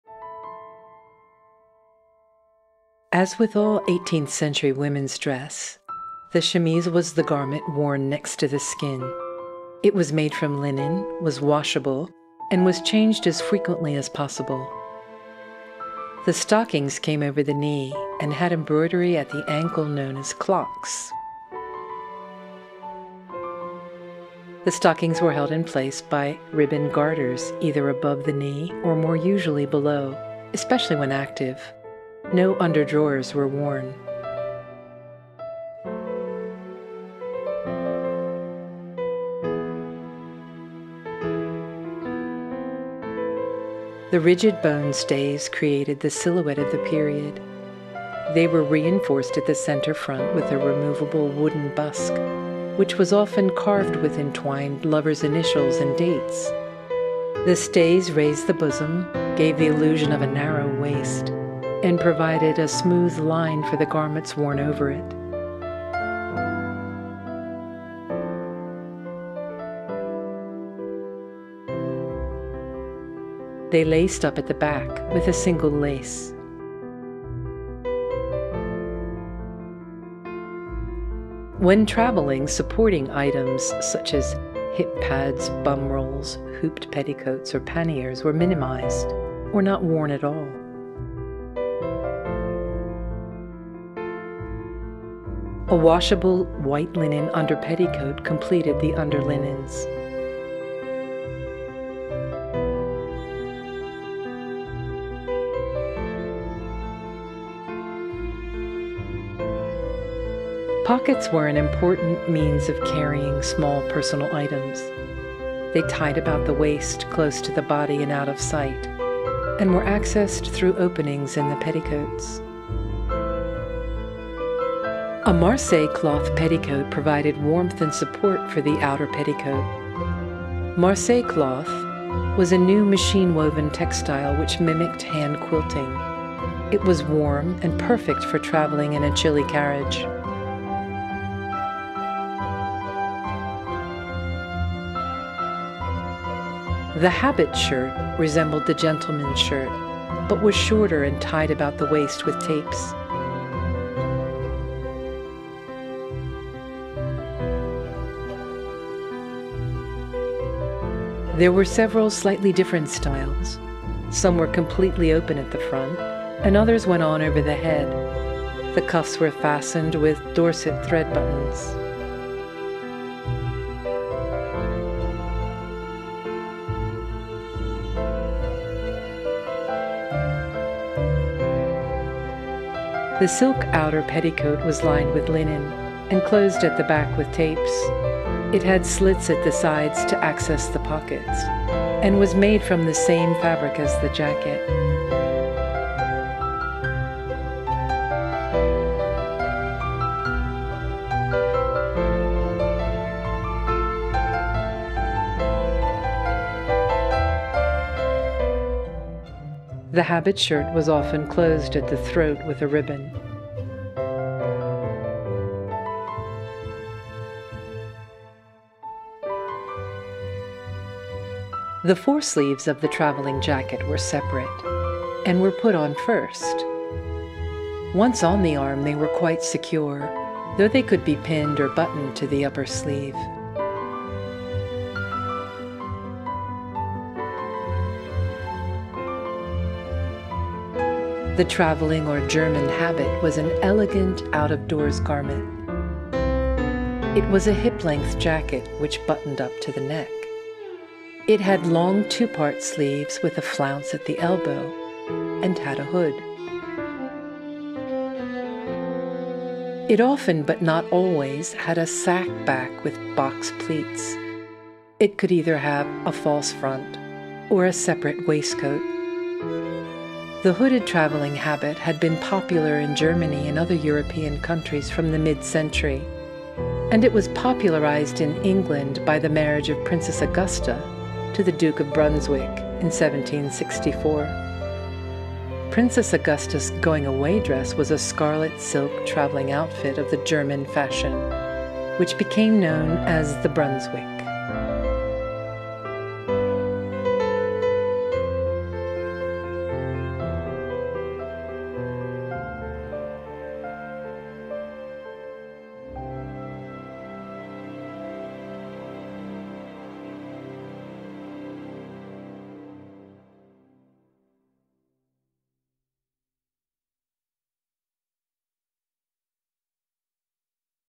Getting Dressed in the 18th Century - The Brunswick
Assisted by her maid, a lady gets dressed for travel in the fashion of 18th century.